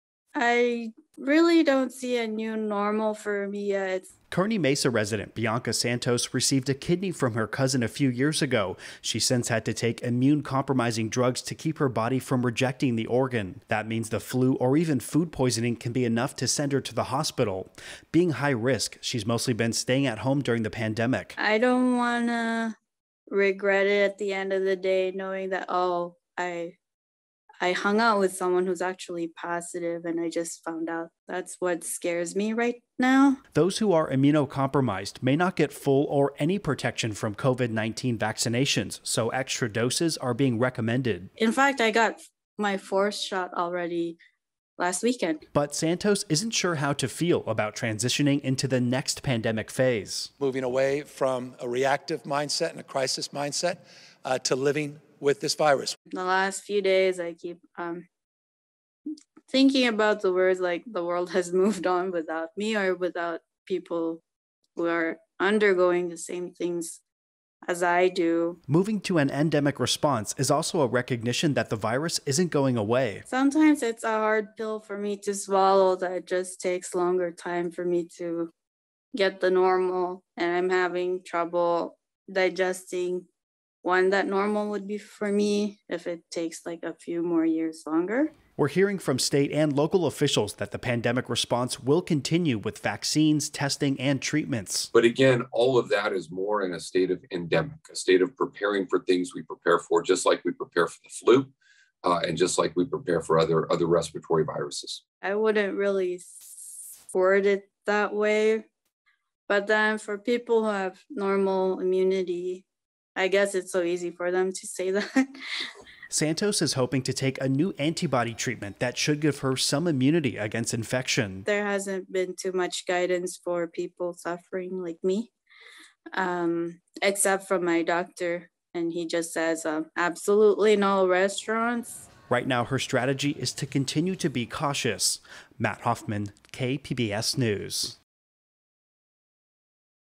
California's endemic phase: not everyone is ready to live with the virus
California has entered a new phase in the fight against COVID-19. But not everyone is ready to ease into the new normal just yet. KPBS Health Reporter Matt Hoffman explains.